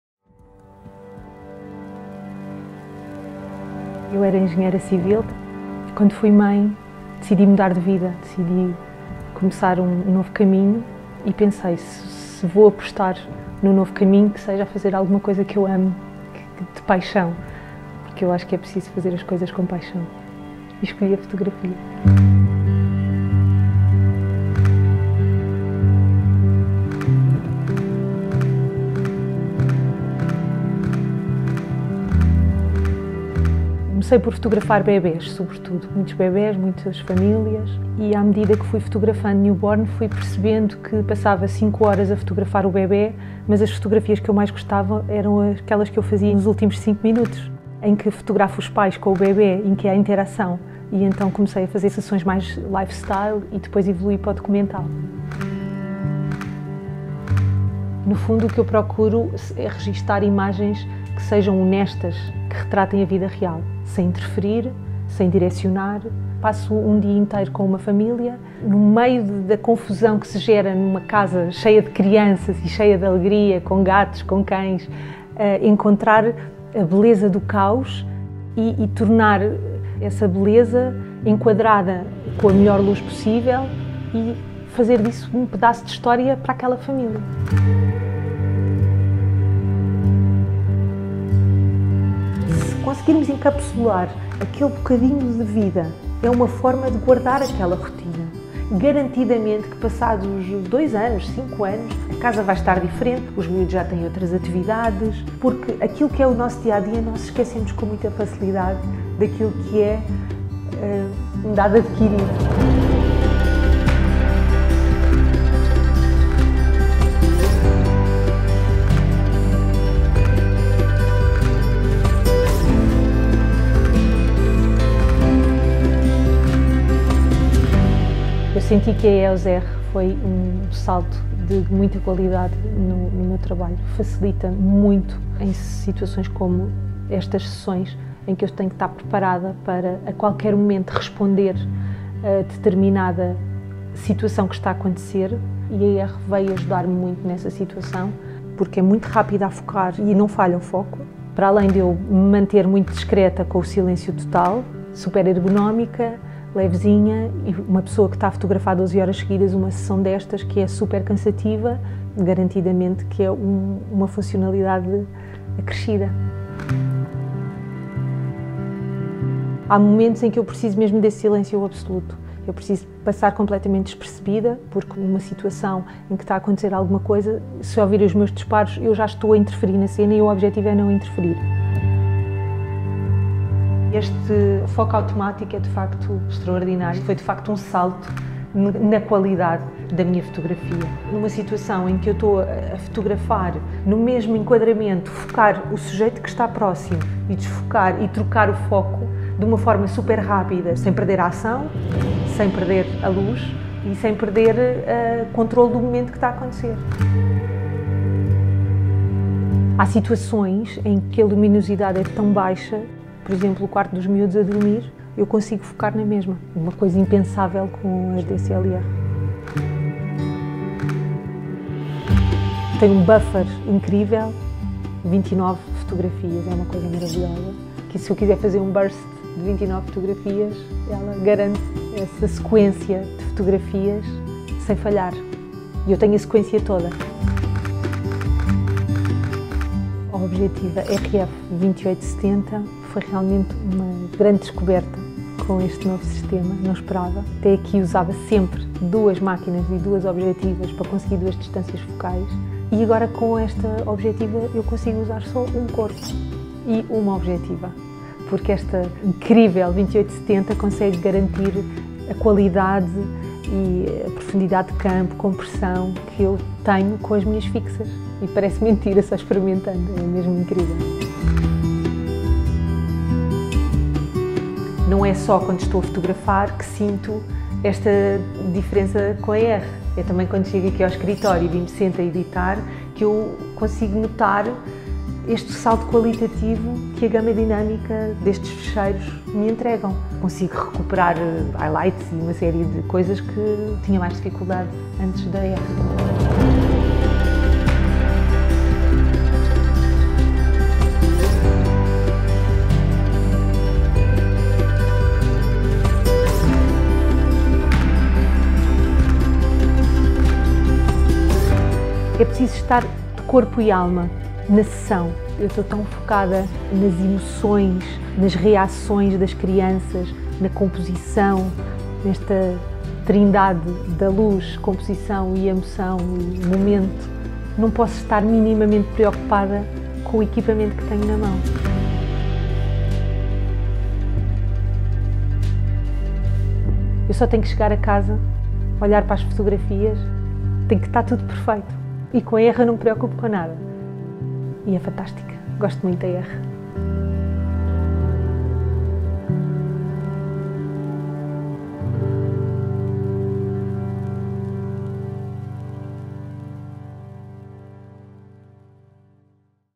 À descoberta do Sistema EOS R com Marisa Martins
Descubra as vantagens únicas do Sistema EOS R na companhia da fotógrafa Marisa Martins e explore como captar os melhores momentos em família com originalidade, preservando memórias inesquecíveis ao estilo de uma fotógrafa nacional de referência.